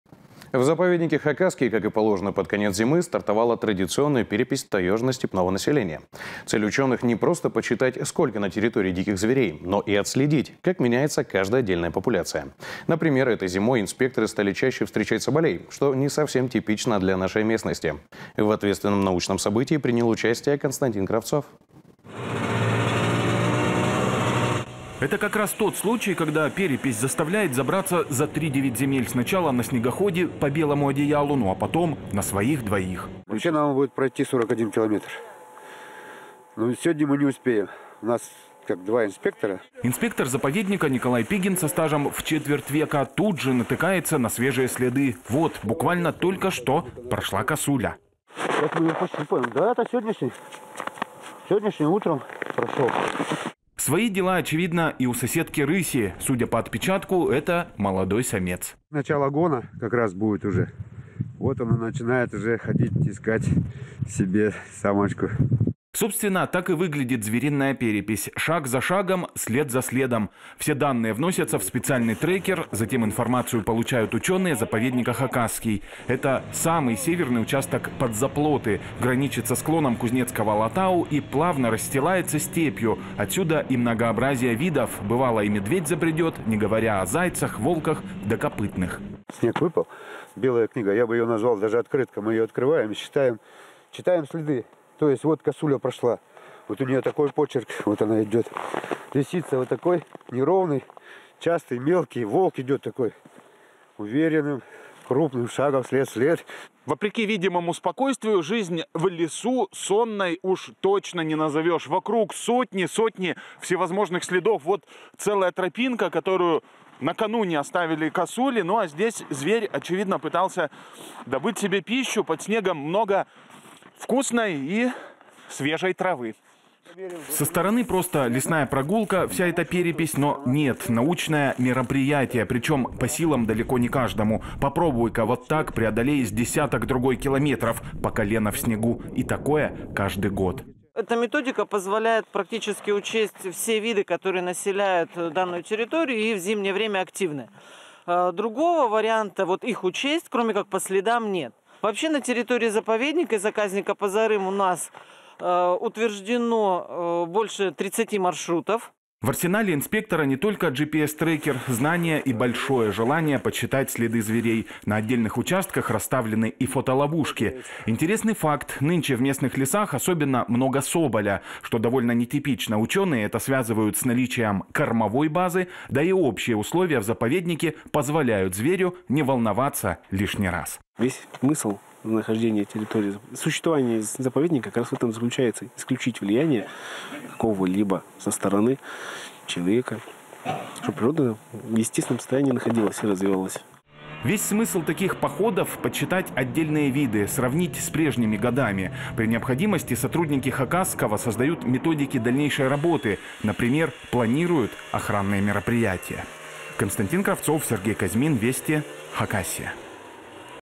В заповеднике «Хакасский» началась традиционная перепись его обитателей
В заповеднике "Хакасский", как и положено под конец зимы, стартовала традиционная перепись таежно-степного населения. Цель ученых - не просто подсчитать сколько на территории диких зверей, но и отследить, как меняется каждая отдельная популяция. Например, этой зимой инспекторы стали чаще встречать соболей, что не типично для нашей местности.